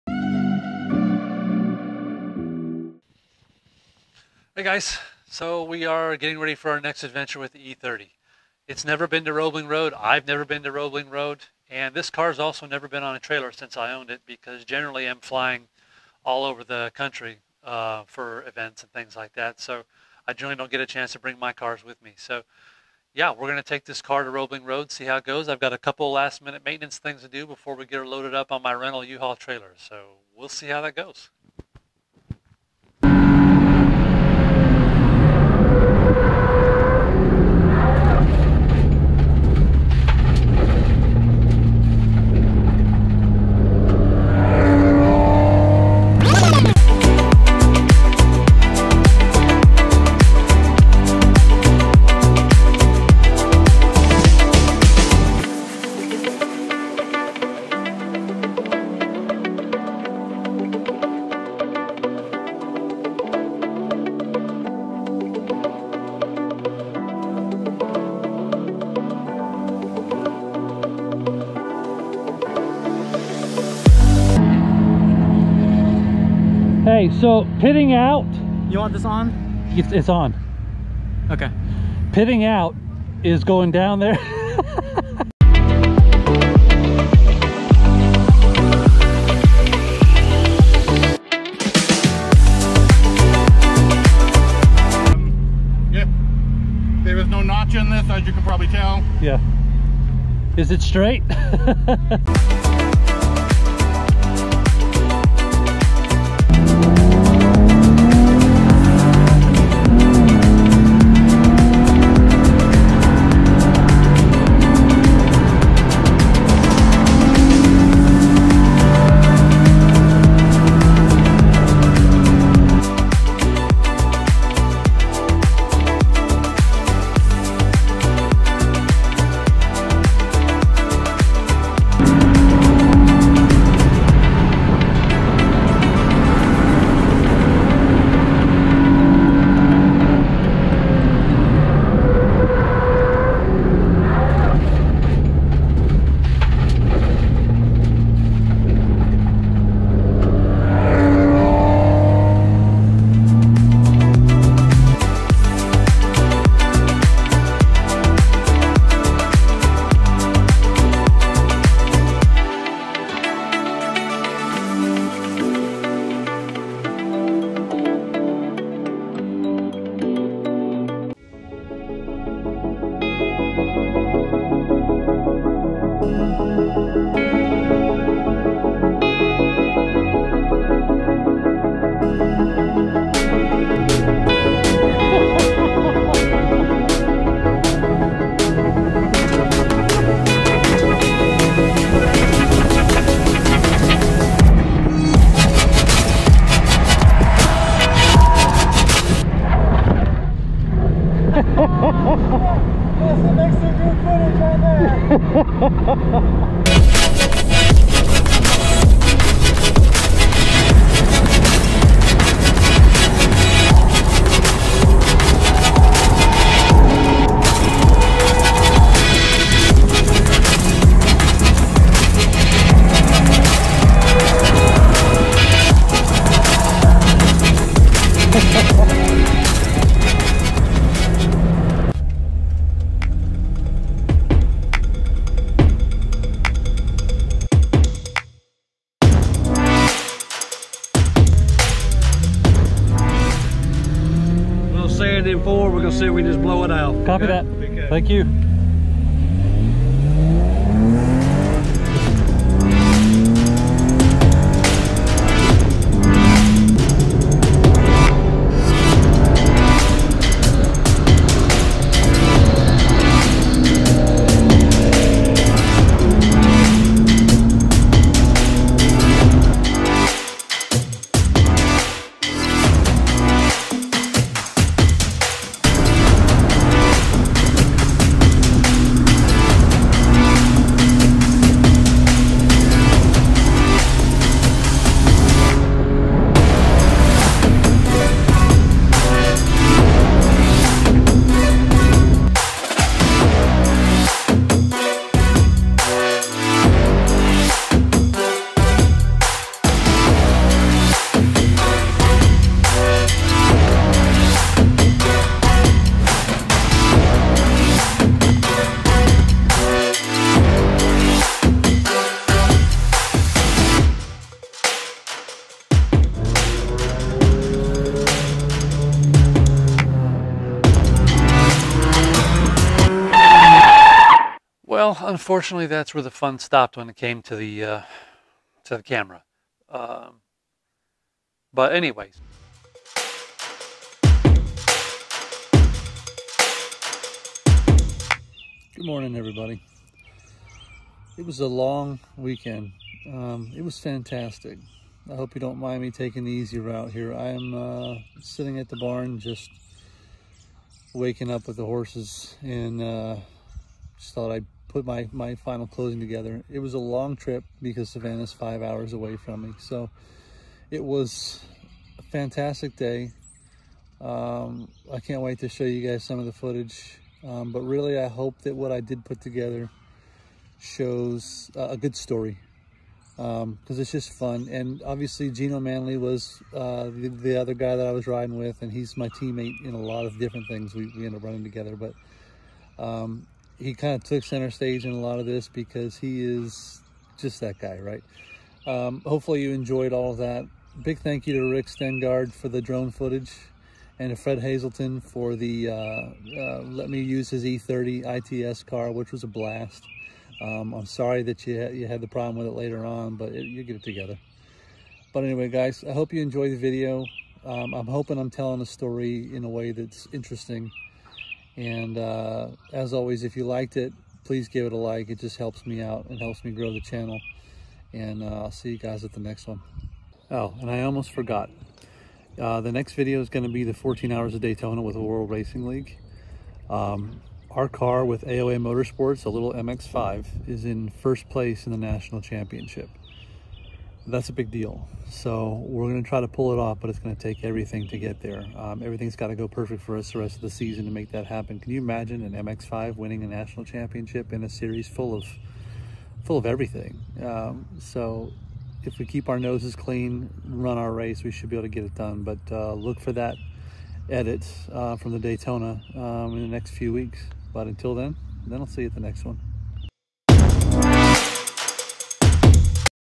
Roebling Road did not quite go as planned...
The story of our track day with Open Throttle Trackdays and AOA Motorsports